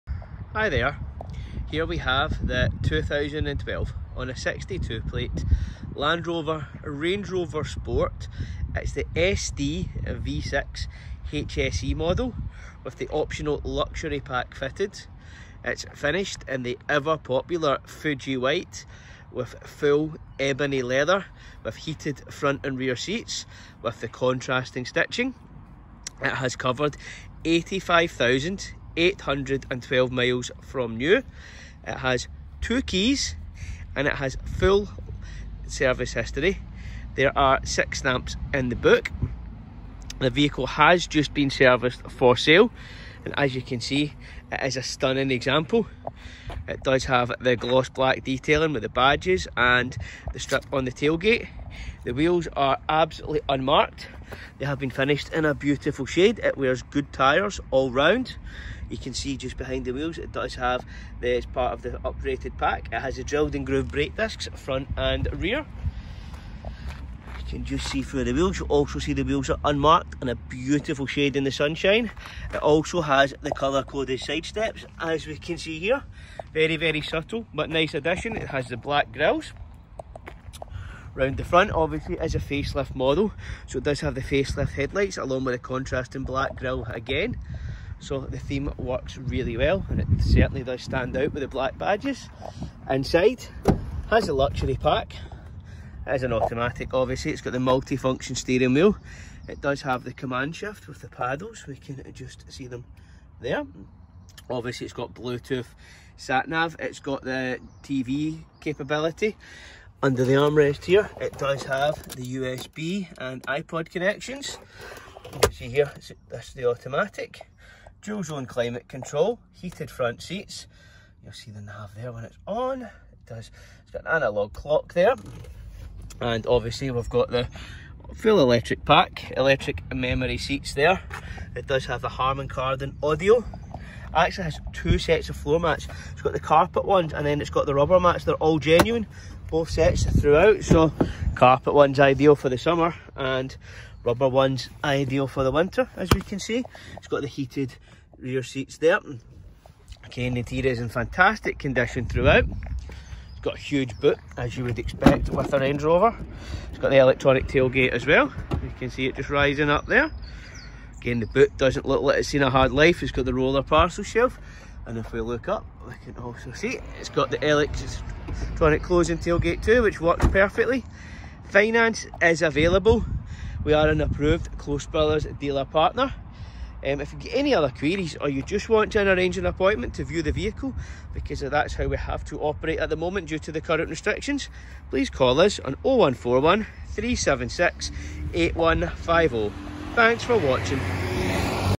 2012 Land Rover Range Rover Sport on sale at TVS Car Sales
Only 85k miles, 2 keys, only 2 former keepers, great service history, just serviced for sale, full 12 months MOT, RAC warranty, full 12 months RAC breakdown cover, great specification!